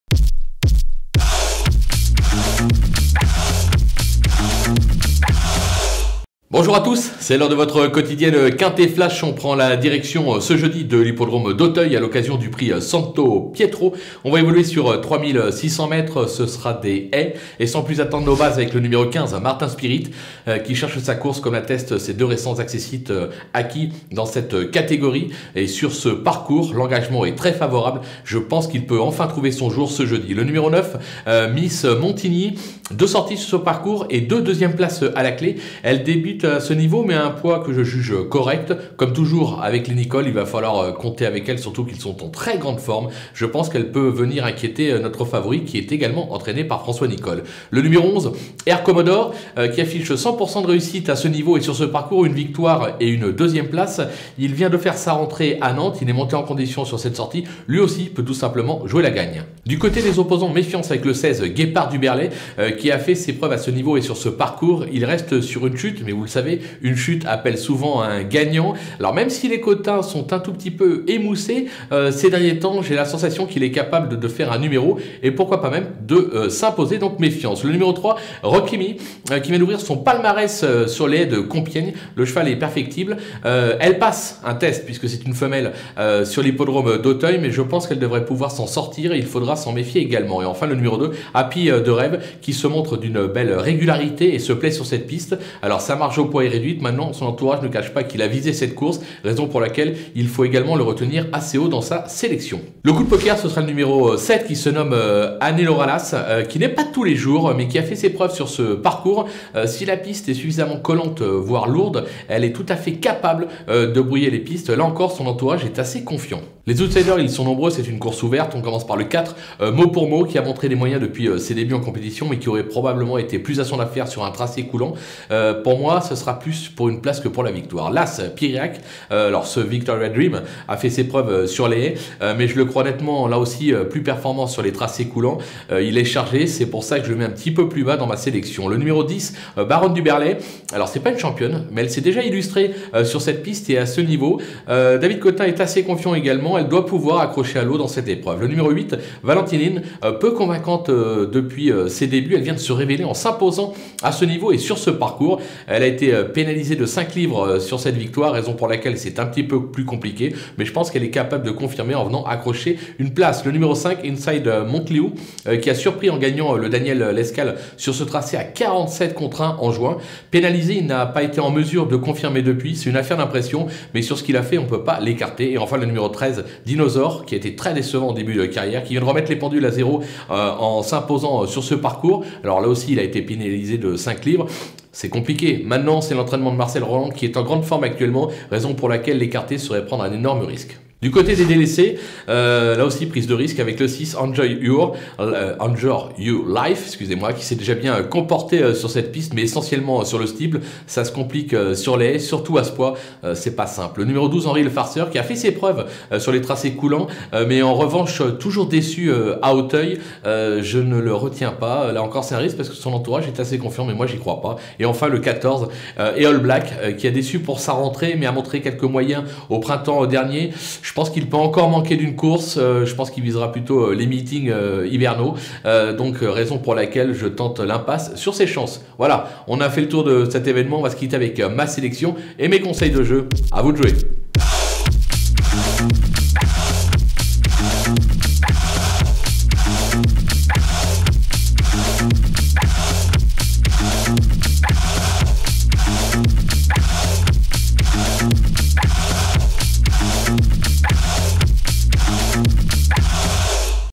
Pronostic PMU Quinté Flash Turf - Auteuil (R1C1 du 21 octobre 2021 - mobile)
Quinté Flash - Philippe Thévenon vous présente les pronostic et conseils de jeu pour la course hippique quinté+ PMU : Auteuil, Prix Santo Pietro (R1C1 du 21 octobre 2021 - mobile). Plus d'infos sur flash-turf.com